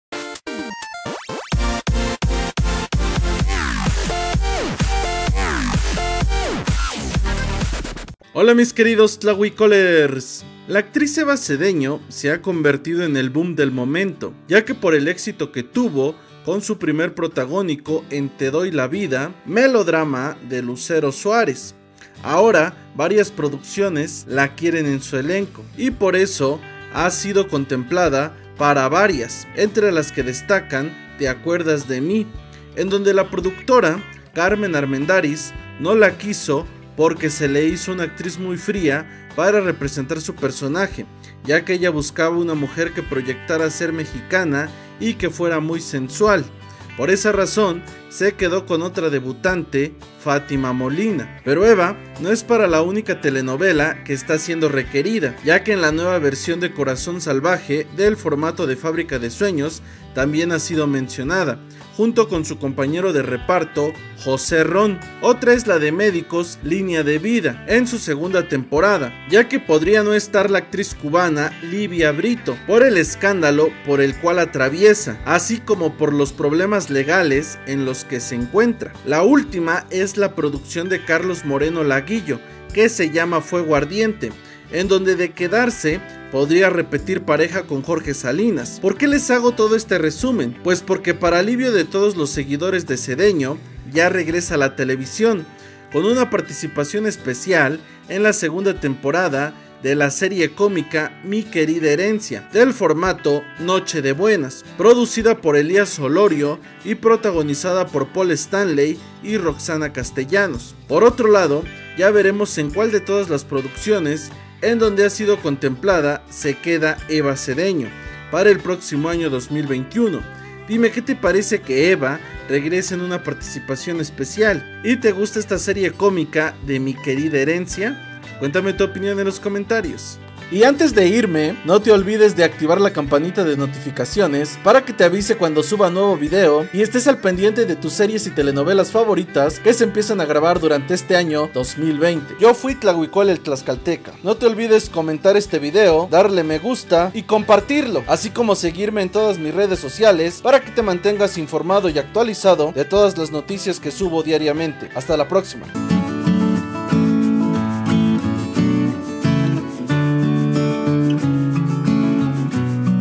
Después de Te Doy La Vida Eva Cedeño regresa con Mi Querida Herencia en su segunda temporada 2020
MiQueridaHerencia en su nueva temporada tendrá a la actriz EvaCedeño quien ha sido muy solicitada en las ultimas semanas gracias al éxito que tuvo en TeDoyLaVida también fue requerida para TeAcuerdasDeMi pero no se quedo, ahora hay otras mas como CorazonSalvaje FuegoArdiente y MedicosLineaDeVida en su segunda temporada en sustitución de LiviaBrito